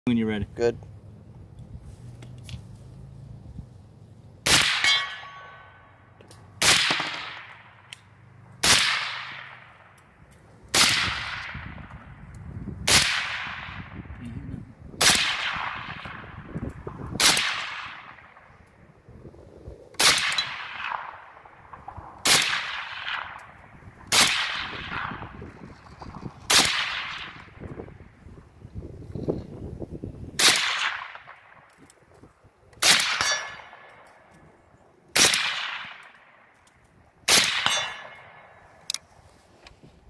Suppressed AK Dead Air wolverine silencer
the Dead Air Wolverine silencer on a Palmetto State Armory AK-47.